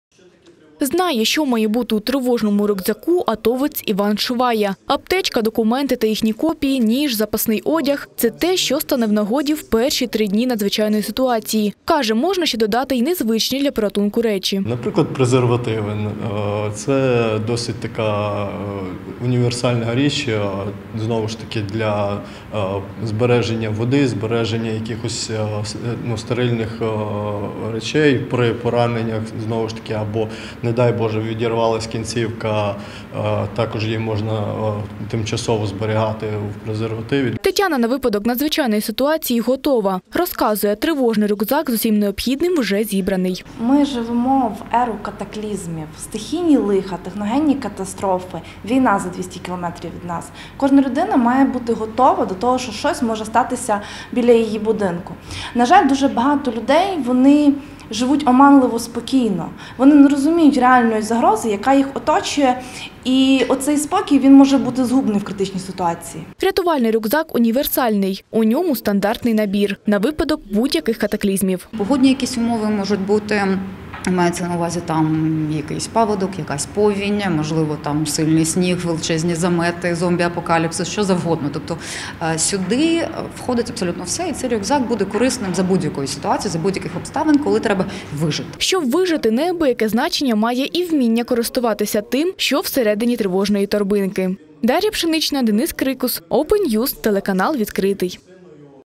Тривожний рюкзак - що це і з чим його їсти?
В їхньому будинку завжди гамірно та весело. Тут живе велика родина Сивак, батьки і шестеро дітей. А ще у домі панують любов, порозуміння і взаємоповага.